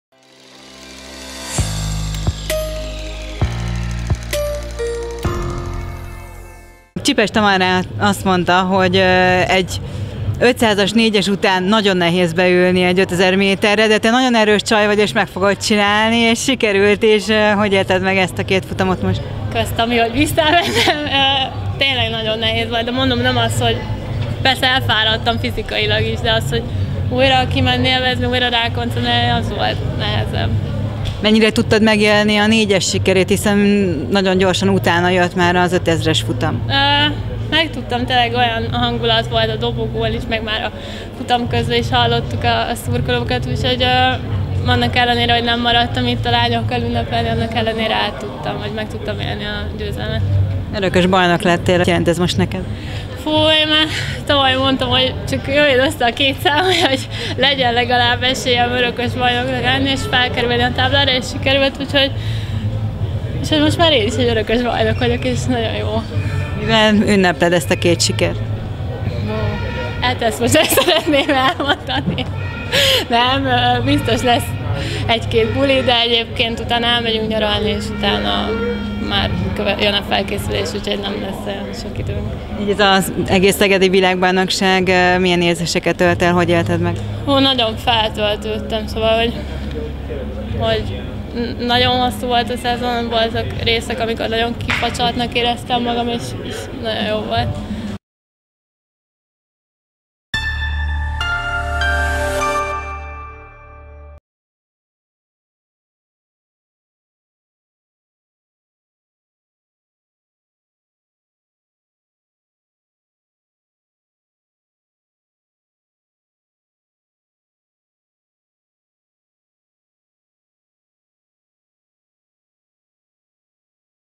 Bodonyi Dóra: Feltöltődtem a szegedi világbajnokságon!
Bodonyi Dóra alig szállt még ki a női kajaknégyes aranyérmes hajójából, máris vízre szállt 5000 méteren, ami után szintén a dobogó legfelső fokára állhatott fel a szegedi kajak-kenu világbajnokságon.